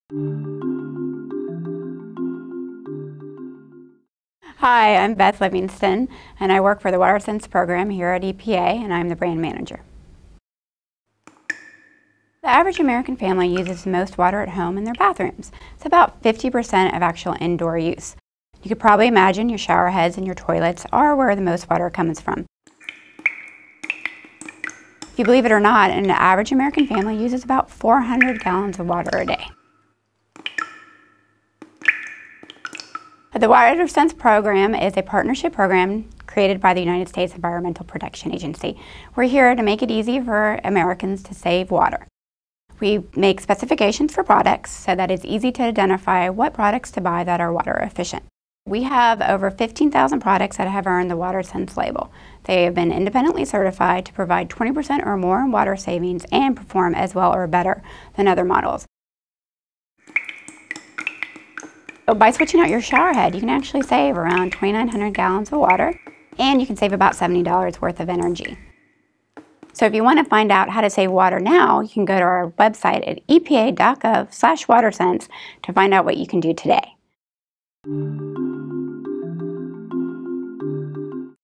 EPA Indoor Water Savings
Water efficiency is the smart use of our water resources through water-saving technologies and simple steps we can all take around the house. Using water efficiently will help ensure reliable water supplies today and for future generations. Best of all, everyone can play their part in preserving our nation's water resources. With our simple steps and informational tools, you'll find that it's easier than ever to save water and money.

Much of our indoor water use occurs in the bathroom. Look for WaterSense products to improve your water use efficiency.